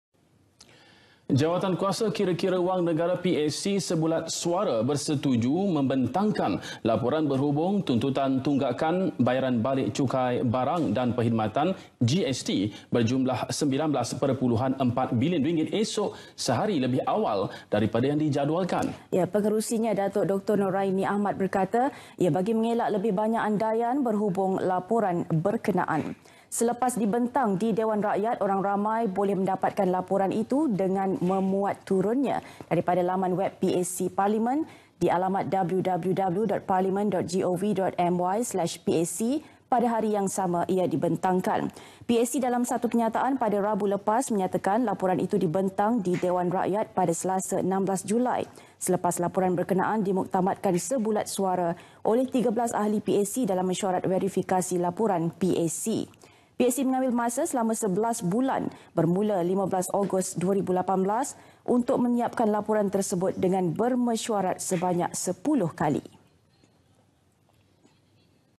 Laporan Tuntutan GST - PAC Bersetuju Bentangkan Tuntutan Tunggakan Cukai GST
Jawatankuasa Kira-Kira Wang Negara (PAC) sebulat suara bersetuju membentangkan laporan berhubung tuntutan tunggakan bayaran balik Cukai Barang dan Perkhidmatan (GST) berjumlah RM19.4 bilion esok, sehari lebih awal daripada yang dijadualkan.

Pengerusinya, Dato' Dr. Noraini Ahmad berkata ia bagi mengelak lebih banyak andaian berhubung laporan berkenaan.

Sumber: Berita Nasional RTM Rasmi